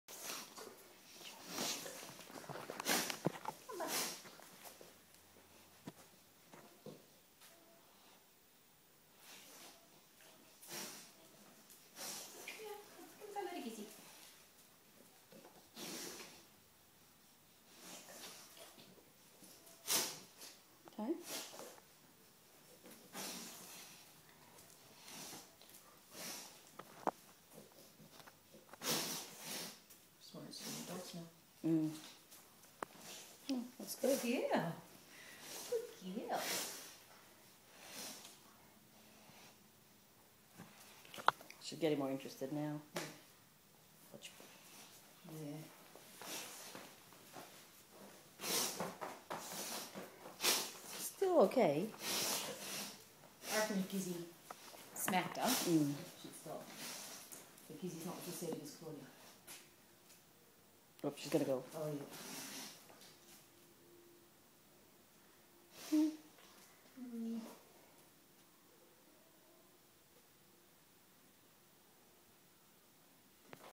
BDR Buffy meets a cat
Temp testing Buffy with a cat. Went pretty well. Buffy is looking for a new home